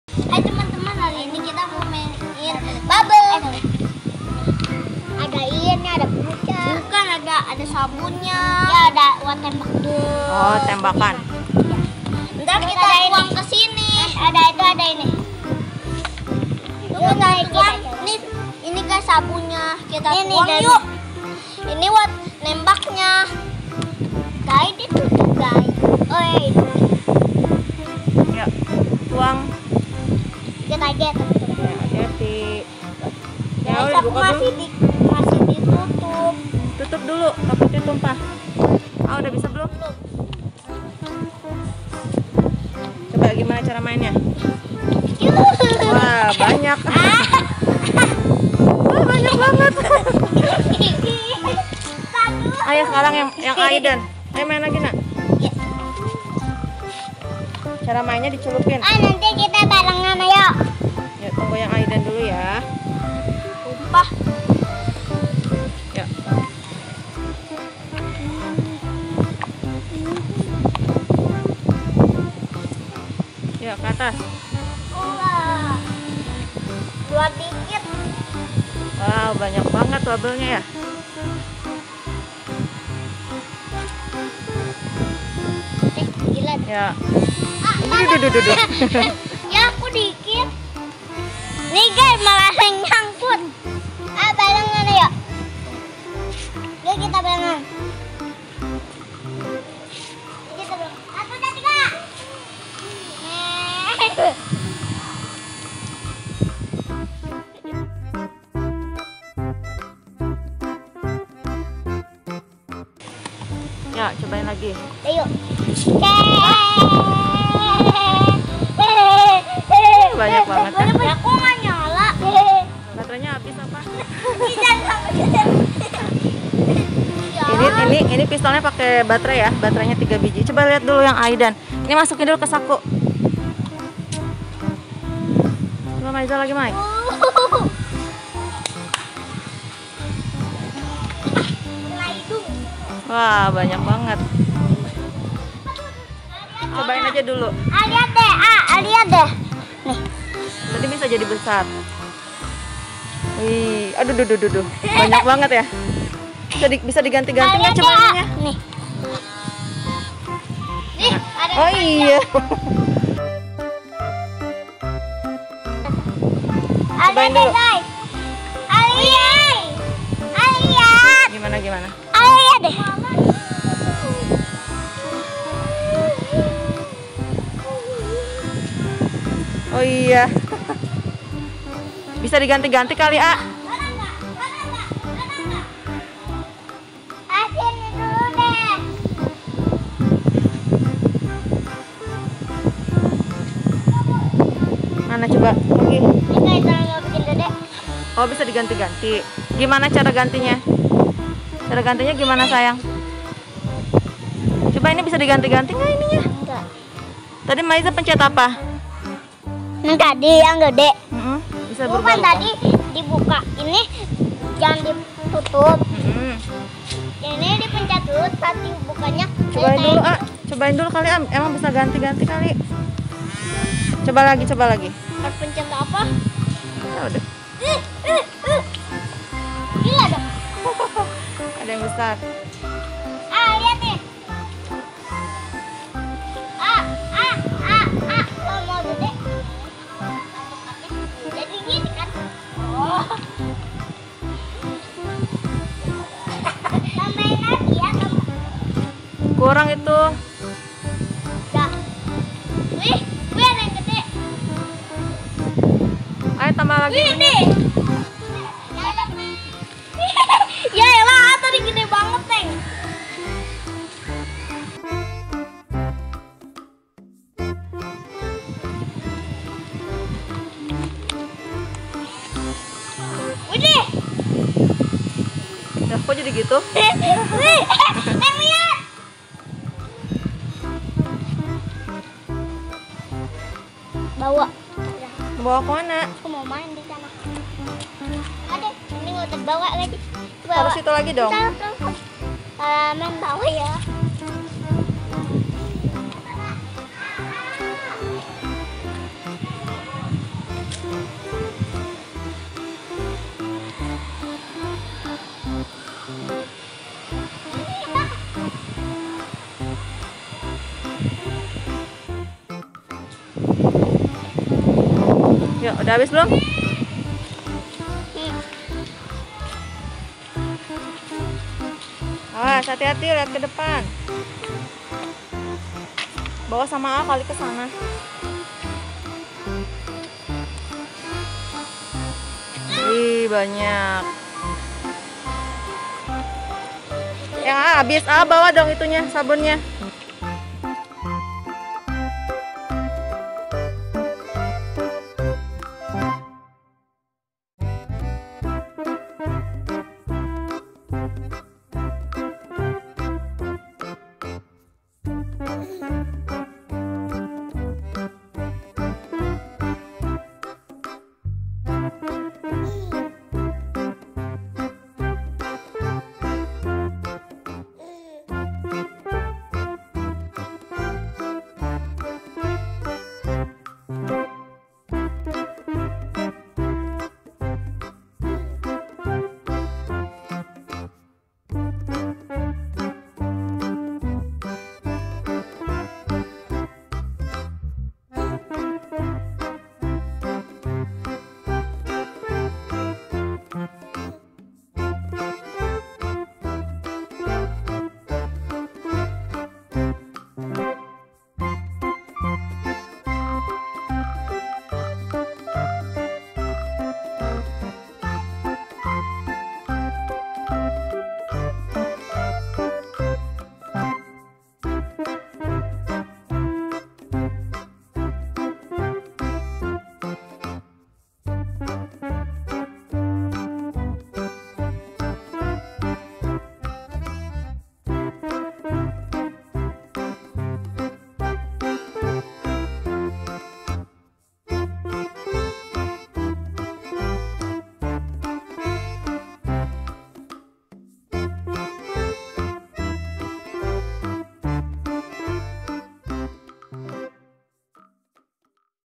GATLING GUN BUBBLE MACHINE
Selamat datang di channel Aidan Maiza Channel, di video kali ini Aidan dan Maiza bermain gatling gun bubble machine.  Pistol gelembung ini mengeluarkan banyak gelembung sabun teman teman, seru banget lho, kalo kalian mau beli mainan bubble machine gun ini, kalian bisa beli di link di bawah ini ya